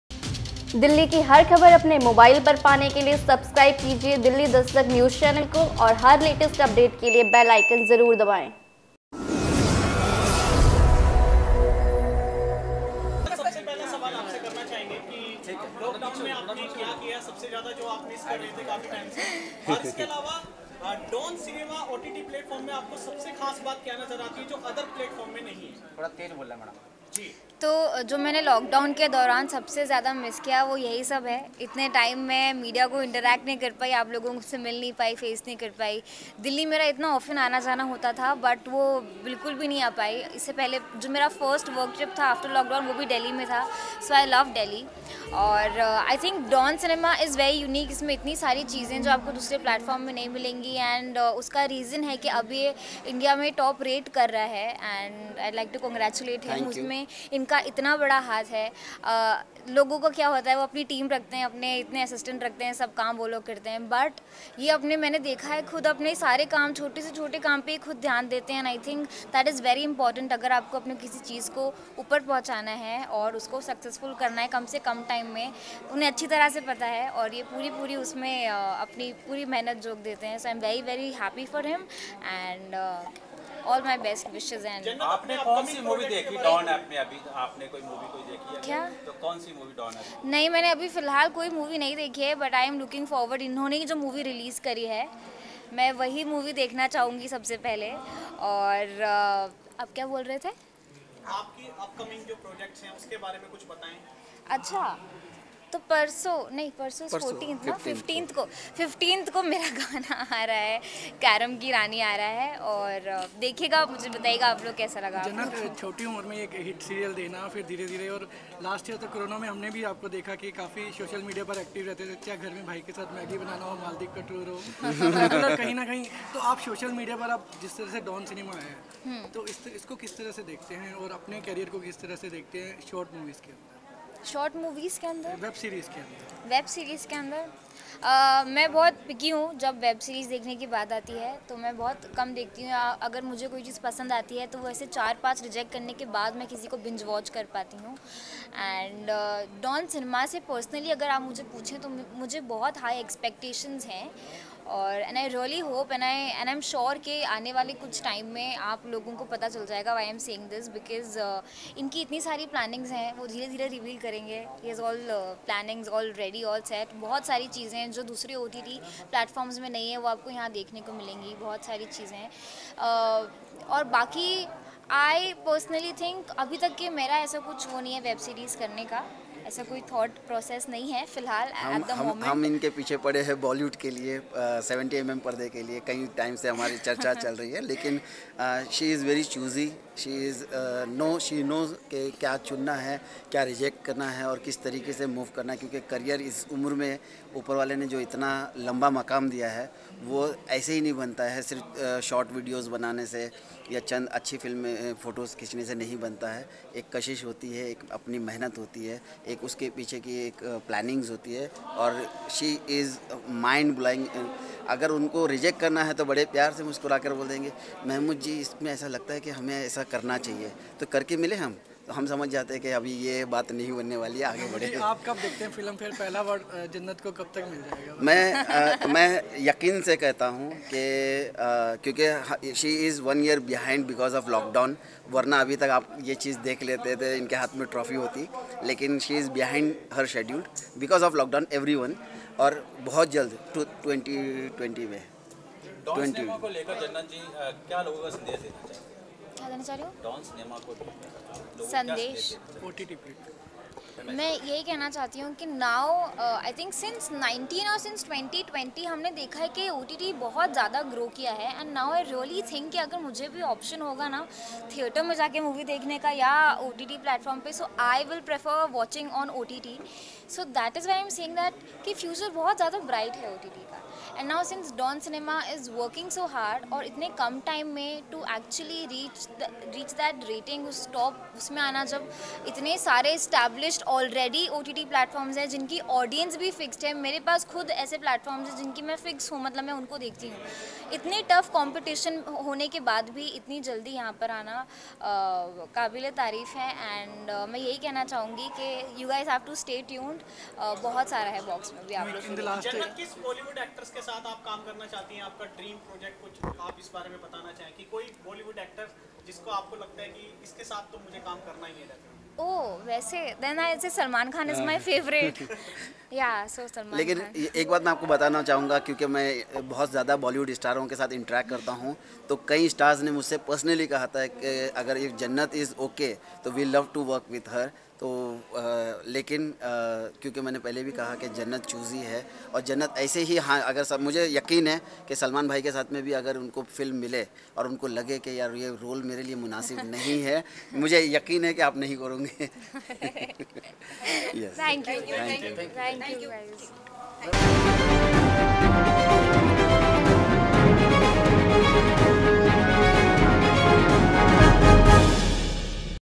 DON CINEMA APP के लॉन्चिंग पर पहुंची अभिनेत्री JANNAT ZUBAIR
Hi

About This Video :- डॉन सिनेमा एप्प के लॉन्चिंग पर पहुंची  अभिनेत्री जन्नत जुबैर